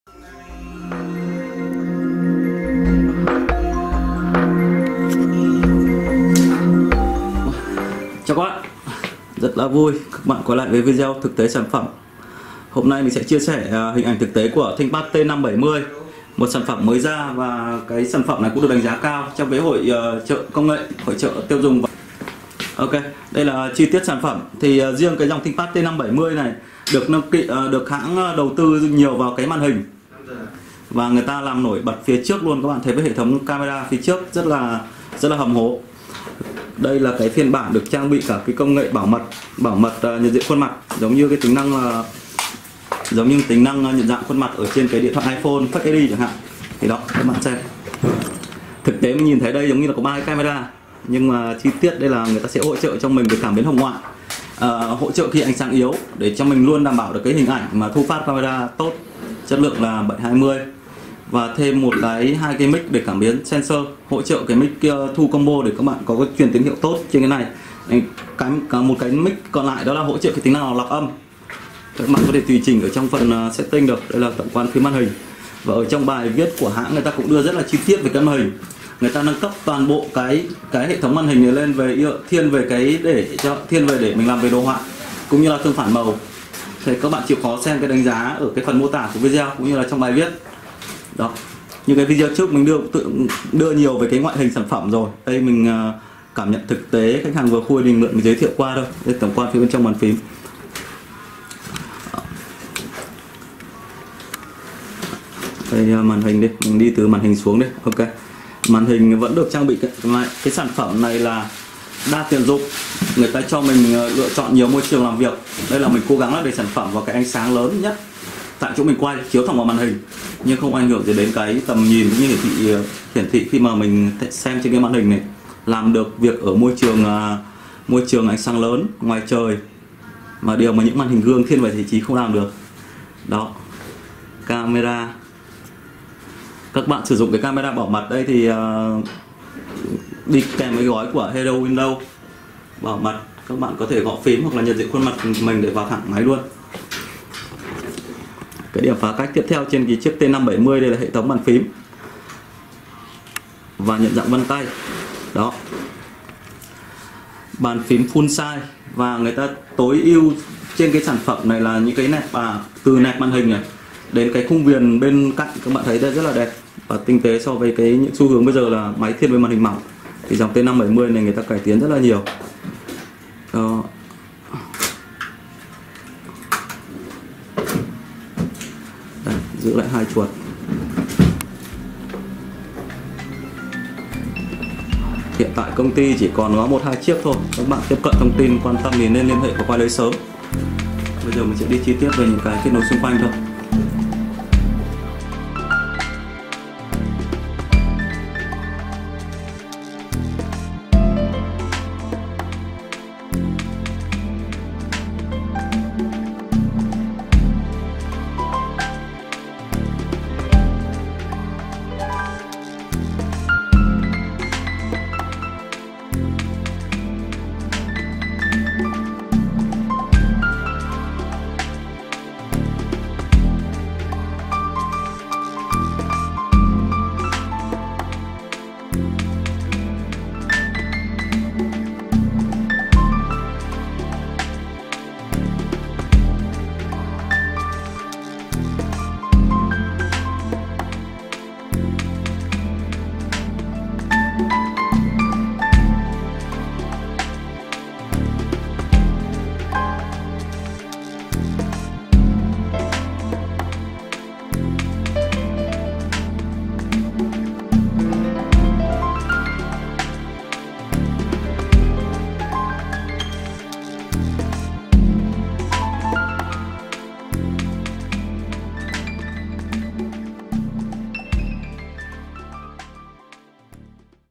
Thinkpad T570 Mới chính hãng nhập Mỹ đã có hàng lại tại Hà nội Lab
Những điểm mạnh được TỐI ƯU trên Thinkpad T570 Khi build ( xây dựng cấu hình ) Đầy đủ tại website nhà sản xuất .

🇱🇷 Nhập trực tiếp từ USA mới 100 % 🇱🇷
💰 Tặng 300$ Là hàng “refurbished” ( Hàng loại 2, loại 3 )

Bộ xử lý Intel Core i7 thế hệ 7 Bộ nhớ lên đến 32GB RAM, bạn có thể chạy đa nhiệm nhiều ứng dụng cao cấp mà không làm chậm máy tính của bạn.
Windows 10 ProWindows
10 Pro được thiết kế phù hợp cho bạn và việc kinh doanh của bạn. Nó cho phép bạn thao tác thoải mái trên thiết bị mọi nơi, mọi lúc trong khi vẫn đảm bảo tính bảo mật và độ an toàn cao. Phù hợp với nhiều mức ngân sách khác nhau và cũng rất dễ sử dụng.

Công nghệ Intel vPro

hiến cho công việc quản lý quy mô lớn trở nên tiện lợi và tiết kiệm hơn bao giờ hết. Quản lý, bảo vệ, sửa chữa, hay định vị những chiếc máy tính xách tay của bạn ở bất kì đâu trên thế giới.

Công nghệ Power Bridge
Kết hợp pin bên trong với một pin bên ngoài có thể tháo rời ngay cả khi máy vẫn đang hoạt động, Power Bridge cung cấp cho bạn thời lượng pin tối đa và linh hoạt. Việc tháo rời pin mà không tắt nguồn laptop giúp bạn có thể sử dụng thêm một pin rời để tăng thời gian sử dụng giữa các lần sạc.

Tùy chọn Dock WiGig

🏠 Hà Nội: Số 144 Giảng Võ - Toà nhà D2 - Ba Đình - HN
🏠 HCM: Số 2A/16 Nguyễn Thị Minh Khai, Đa Kao, Quận 1